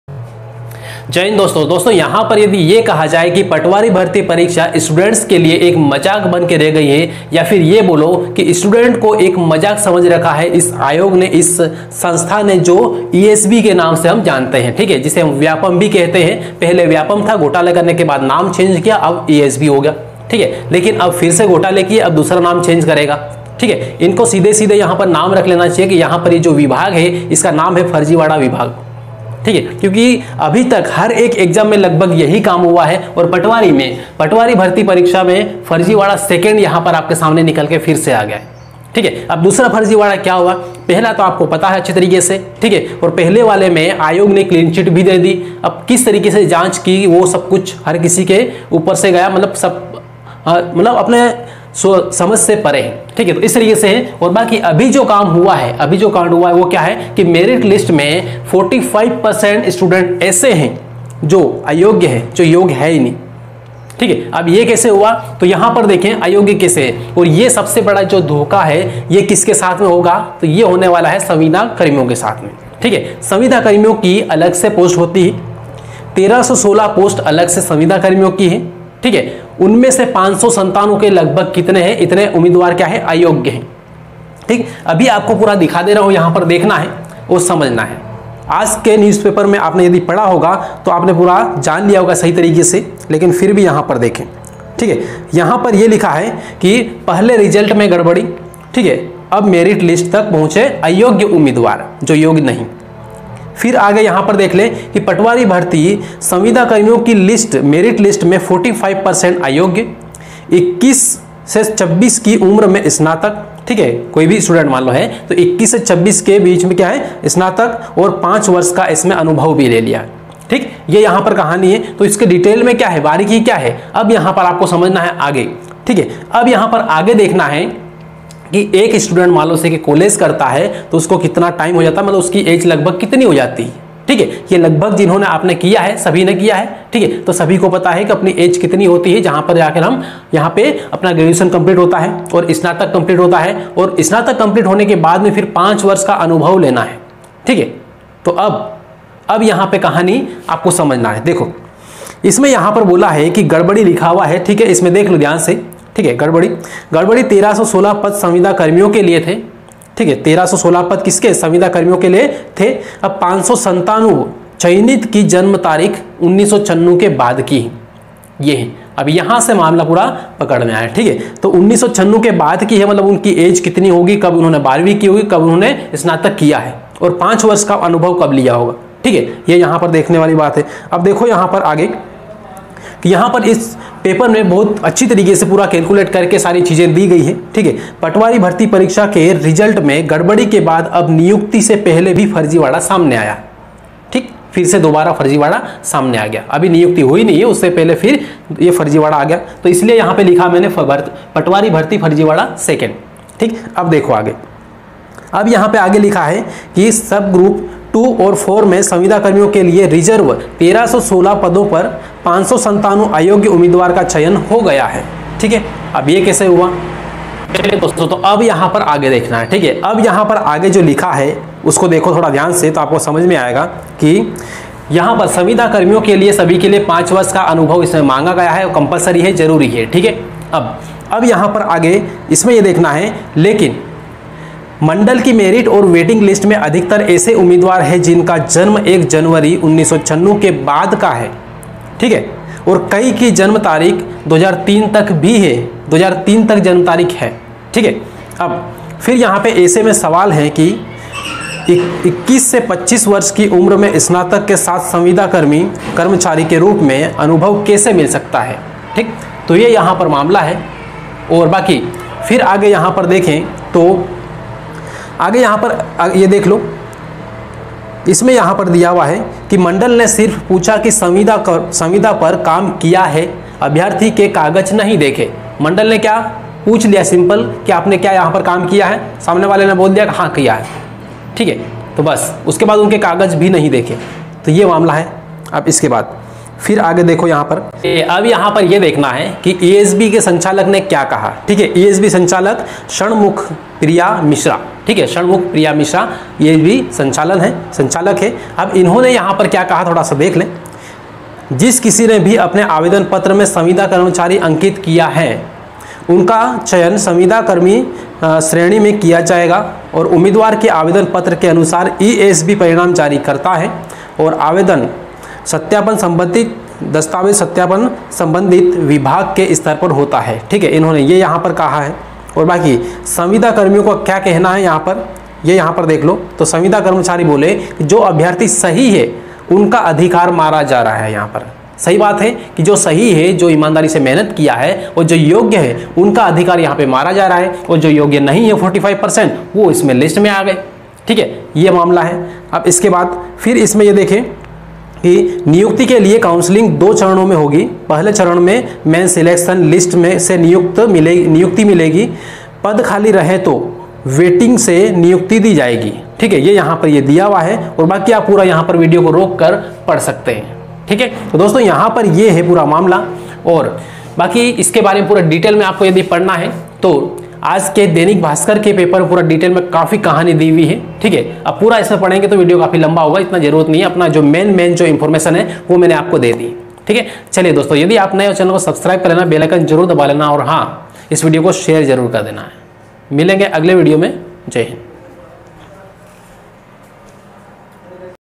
पटवारी फर्जीवाड़ा- 2 || Mp Patwari Result Farjivada || Patwari Result Big Update
About Video :-
 Patwari Bharti Mein FIR hua farjivada. Samvida karmchariyon Ko Hoga sabse bada nuksan. Patwari Merit list mein Aayog ke ummedwar. Aayog ke ummedwaron ka selection Aur yogya ummedwaron Ko Bahar Kar Diya.